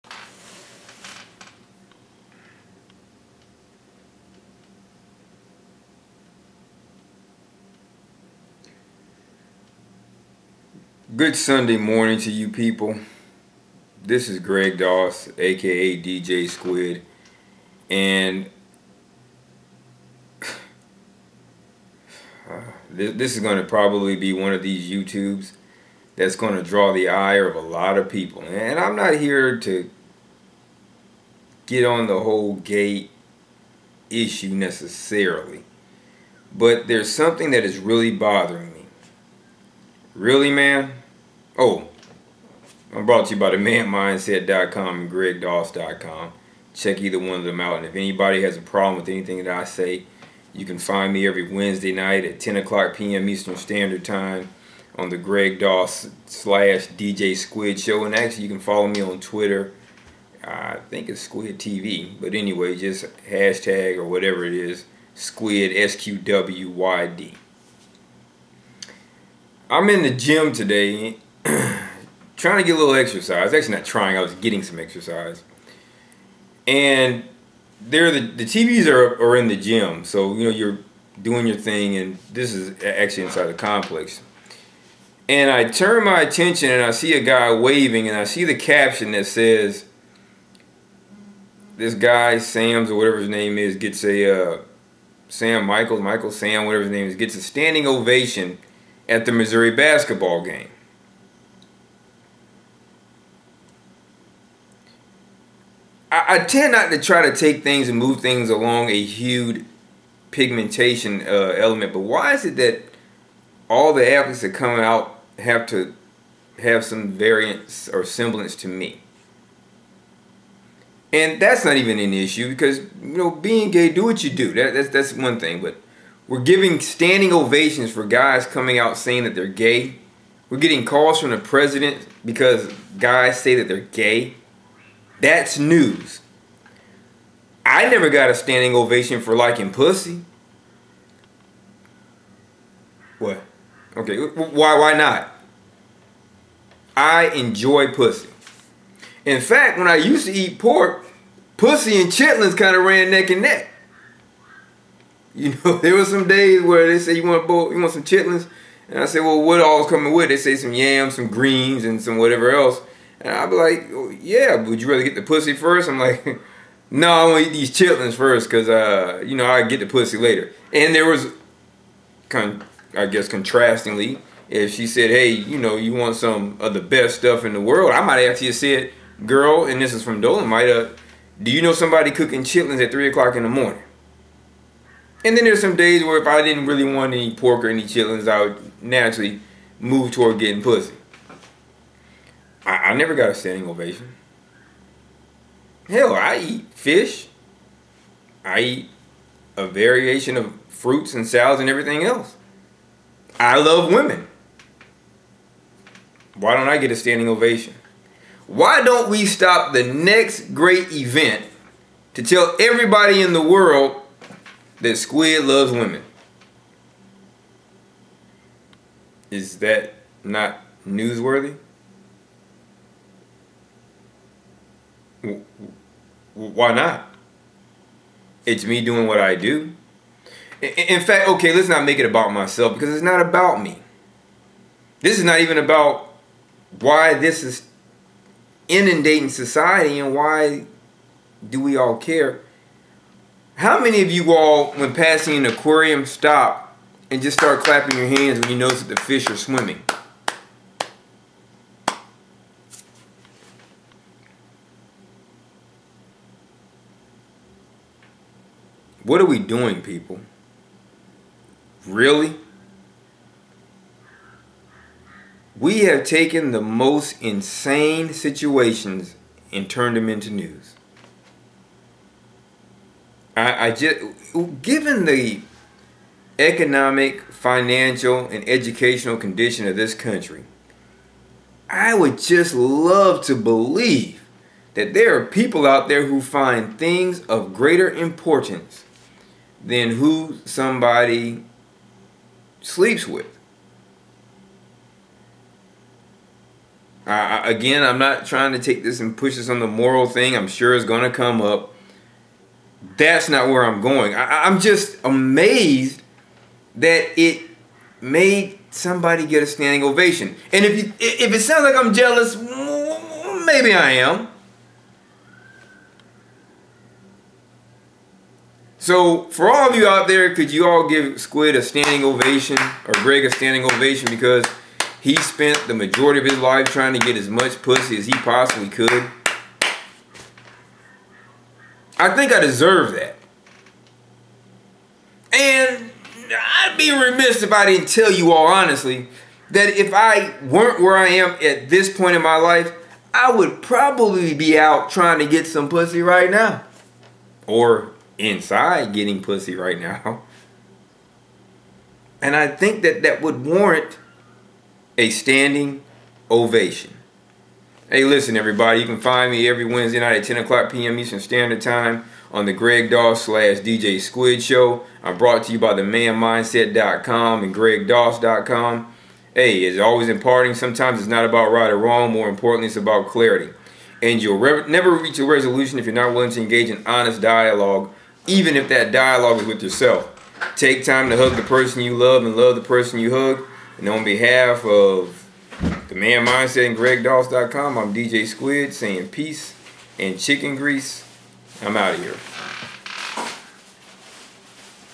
A STANDING OVATION FOR BEING GAY? REALLY, IS THAT WERE WE ARE TODAY?
Why is it newsworthy to admit to being gay? Why was this a center-stage incident at a college basketball game? Ya know, I never got recognition for liking women? .